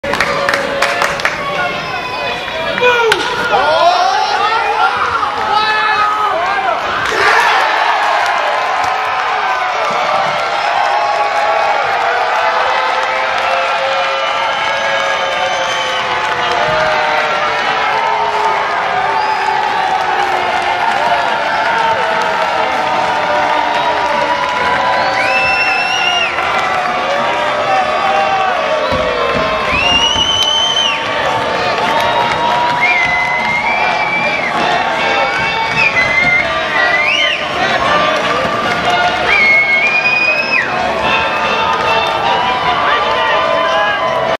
New Hampshire Fisher Cats no-hitter against Portland Sea Dogs
Fisher Cats pitchers Brett Cecil, Danny Farquhar and Ron Uviedo combined on a no-hitter to beat the Portland Sea Dogs, 6-0, on May 21, 2012 at Manchester, NH. For the final out, Uviedo induces a grounder off the bat of Bryce Brentz. Ryan Goins makes the throw to Mike McDade to clinch the third no-hitter in Fisher Cats history.